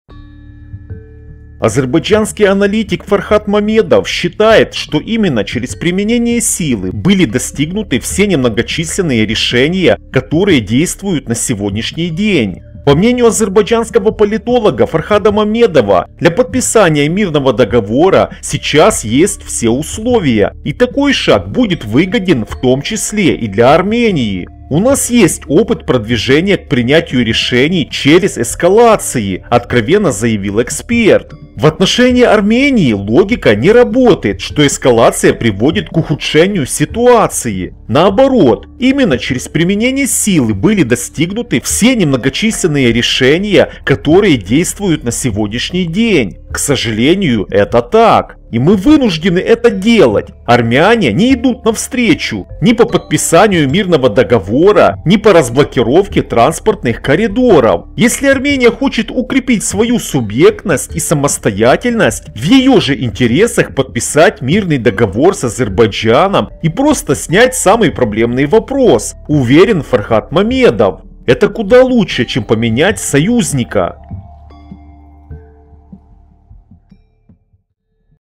Армении доходит только через приминение к ней силы
Мнение руководителя центра исследований Южного Кавказа Фархада Мамедова